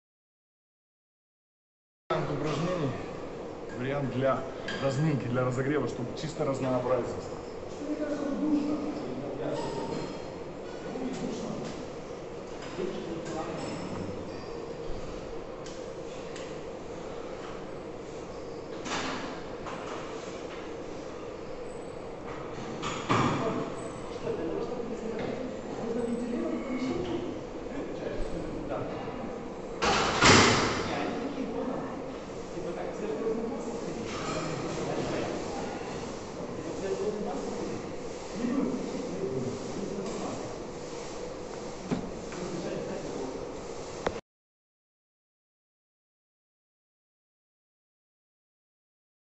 Разминка-разогрев. Плечевой пояс. Гантели.
Вариант комплексного упражнения с гантелями для разминки и разогрева плечевого пояса.
Вес подбирать с расчётом на 10 - 12 раз на каждый комплекс )